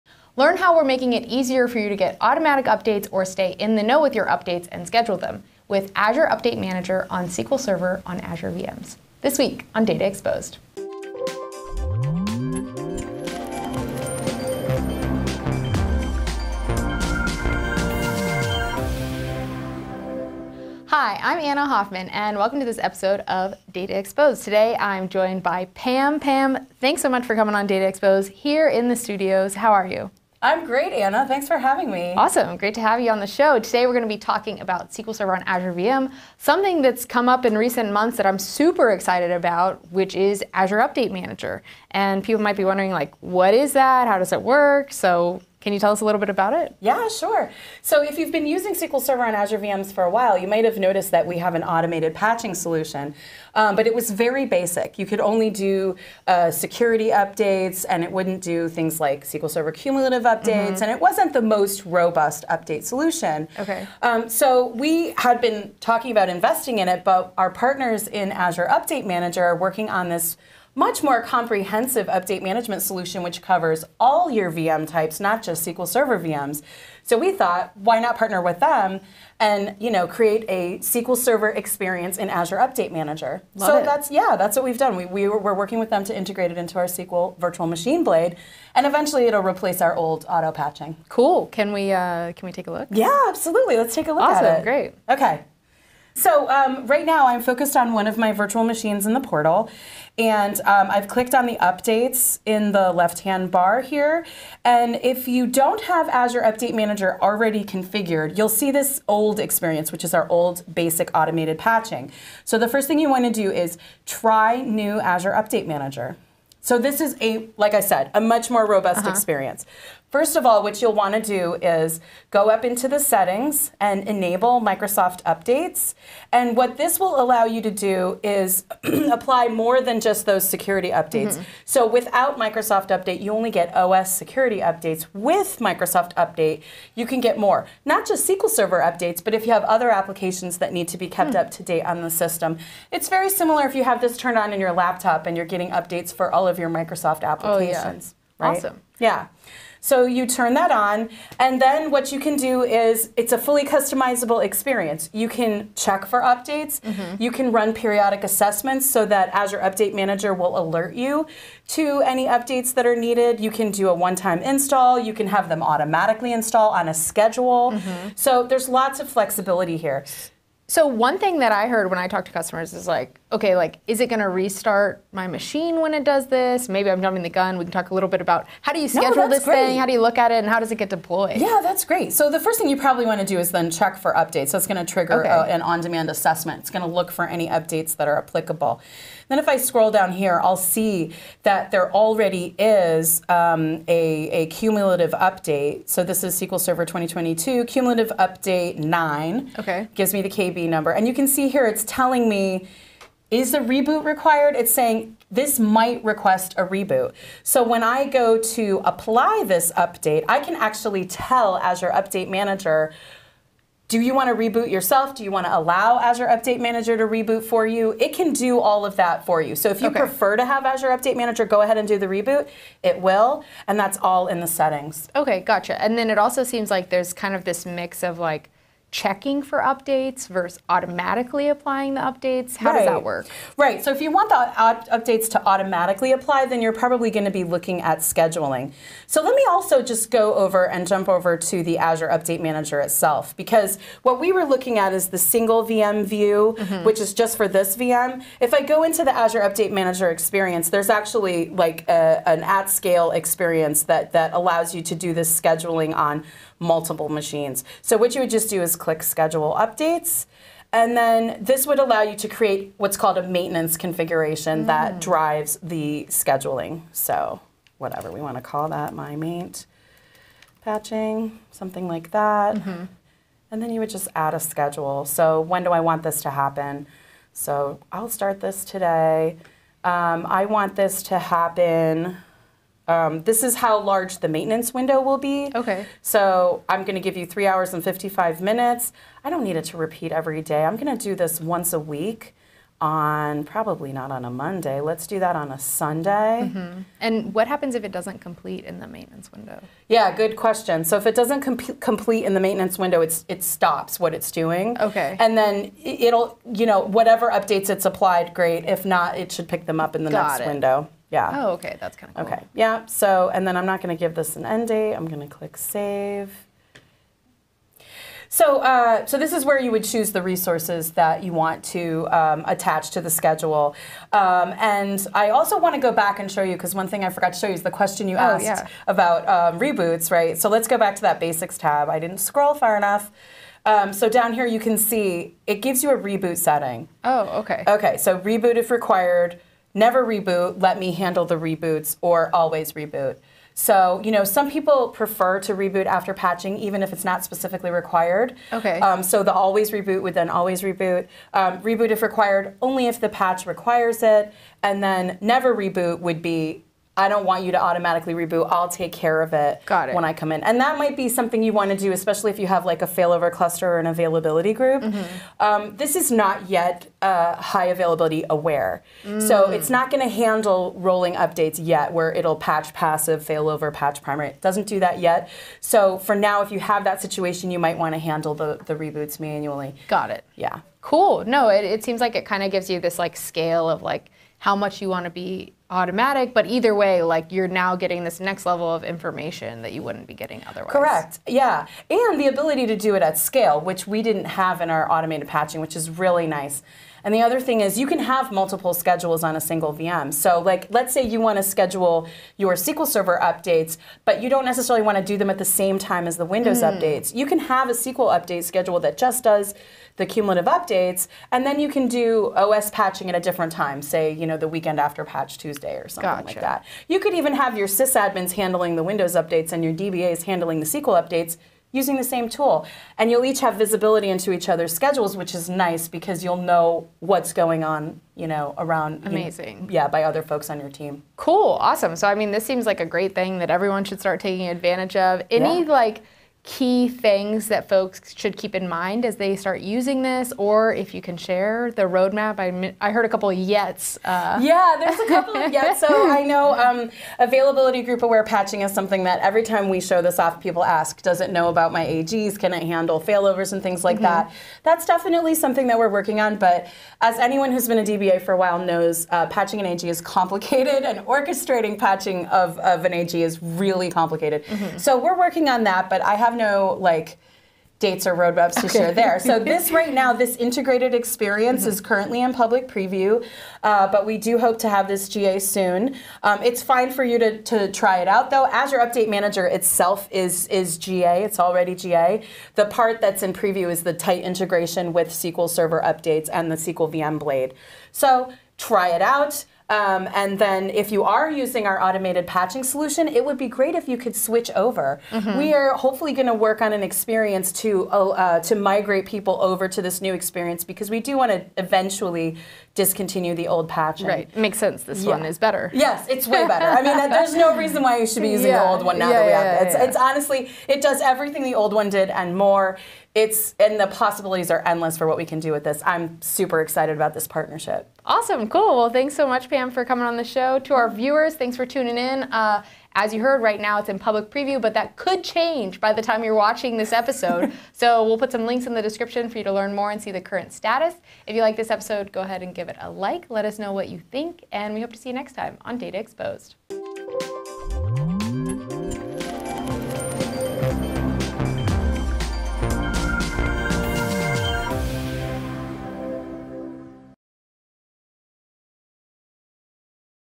A new way for automatic updates with SQL Server on Azure VM | Data Exposed
Learn how to stay up to date with Azure Update Manager on SQL Server Azure VMs.

Chapters:
00:21 - Introductions
00:45 - Azure Update Manager Explained
01:34 - Demo of Update Manager - turning it on, and enabling the updates
02:50 - Scheduling, assessments, and deployments
03:35 - Configuring the settings
03:52 - Automatic versus manual updates
04:10 - Updating multiple machines
06:01 - Settings
07:18 - Further thoughts and roadmap
08:40 - Closing